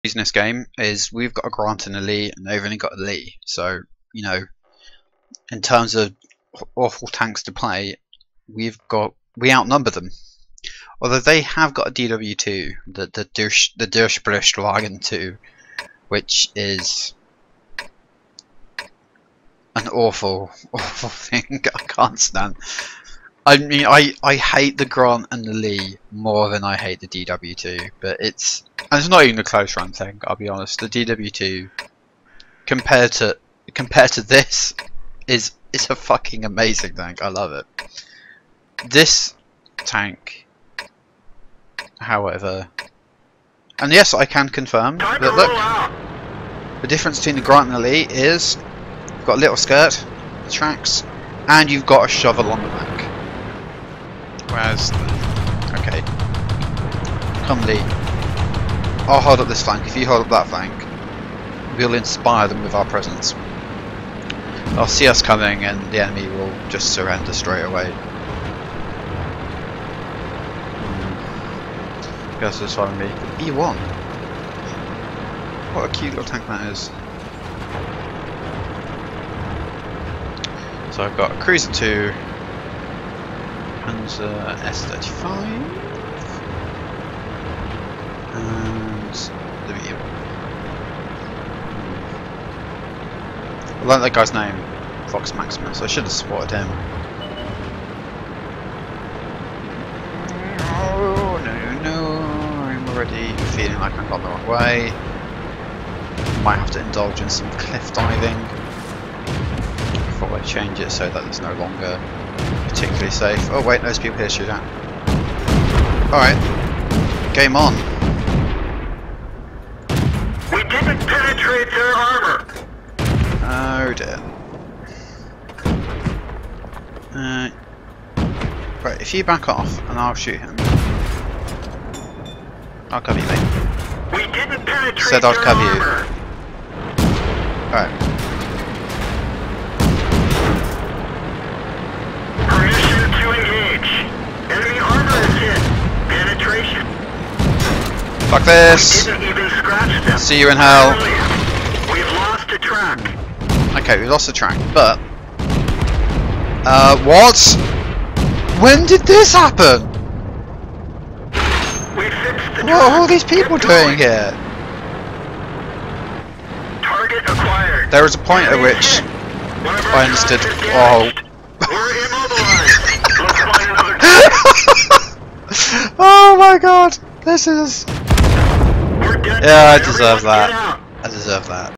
A Tank A Day 14 - Live Cliff Action
Live recording from a game, so may contain some naughty words. The impossible mission to grind through the Grant and on to the sweet green pastures of the Sherman III.